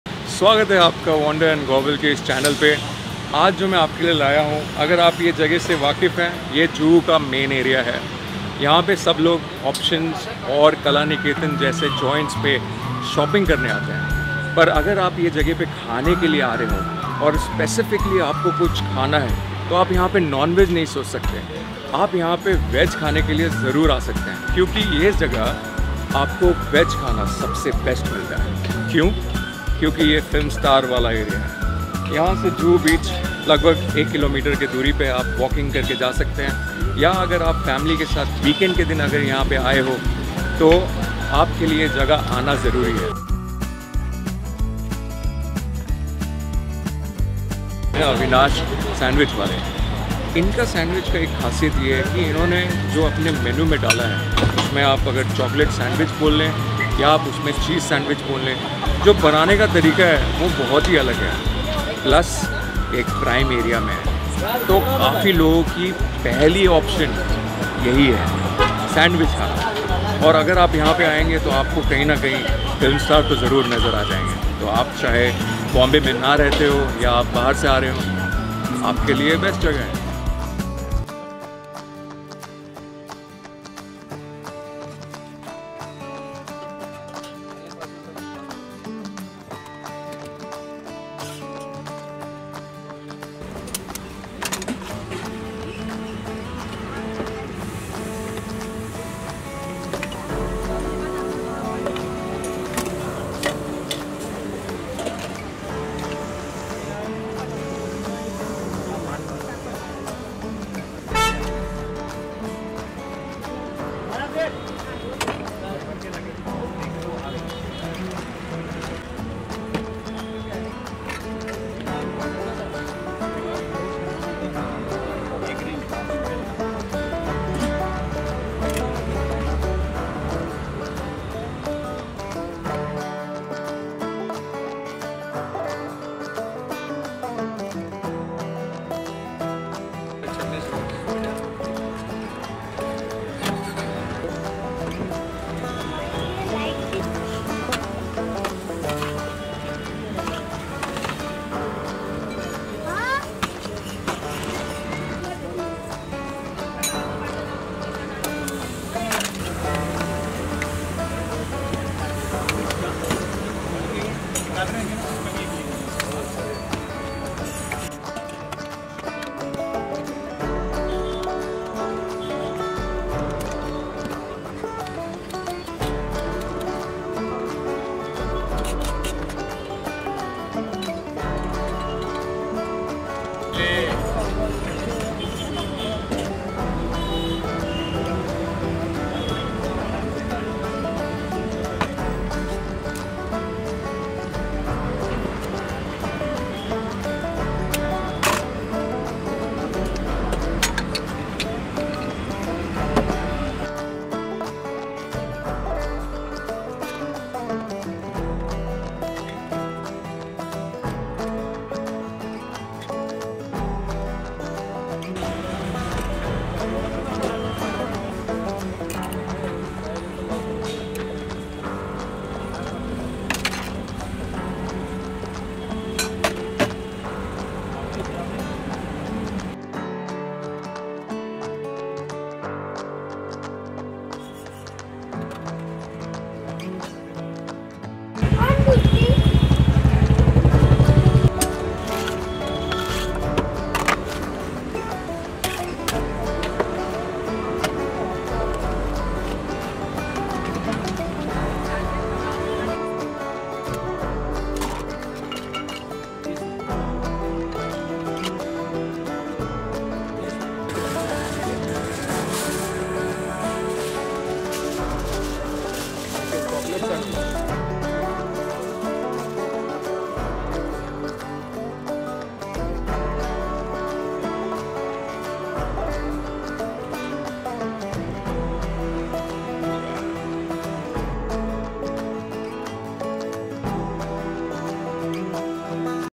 Juhu Ka Sandwich King -Bollywood Filmstar ki galli - Paparazzi Hot spot
The Paparazzi Name one Mumbai Hot Spots You’ll Always Spot A Celebrity here just before Juhu's main lane where superstars stay, 400 mtrs from Amitabh Bachchan’s Bungalow Jalsa. for street food, they all come to this spot for sandwiches, dosa, juices, and paan. one block away from Options and Kalaniketan showroom, Avinash Sandwich has been famous for 2 decades here. visit them during evening time with family and friends and get the best delicious sandwich as quick bites. Mumbai foodie delight has no options to go to this hotspot and relish their taste bud.